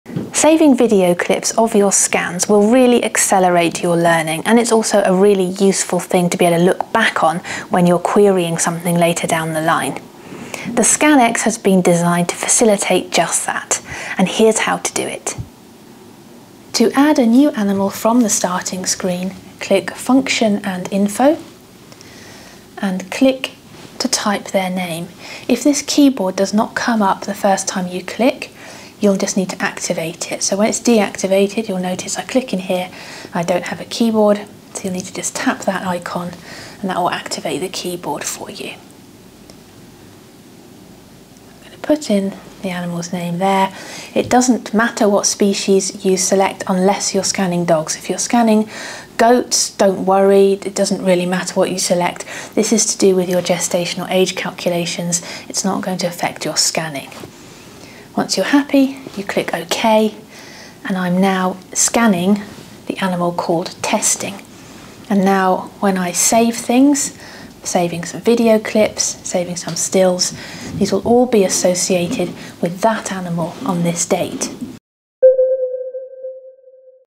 How to quickly add your animals into your ScanX, and make scanning easier
Here's how to keep yourself organized from day one, making life quicker and easier in the long run!

Thank you to Trish for requesting this video.

The bruise I am sporting in this video was acquired by hitting myself in the head with the probe. If you would like a walkthrough on this or any other things you can do with your ScanX, please request in the comments below.